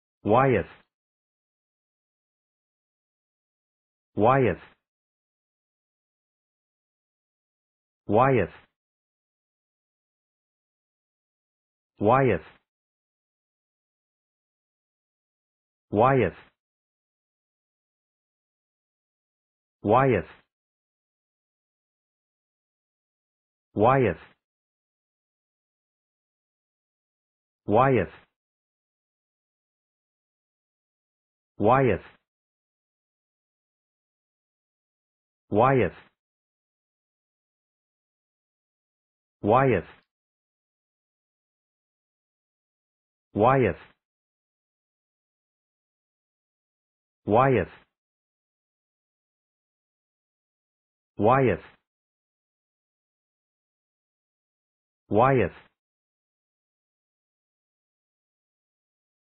Wyeth pronunciation and definition
Learn how to pronounce Wyeth in English correctly - pronunciation of Wyeth
Click show more for definition of Wyeth
WYETH
Pronunciation: /'waɪəθ/ - wy-eth
Definition: (noun) United States painter (born in 1917)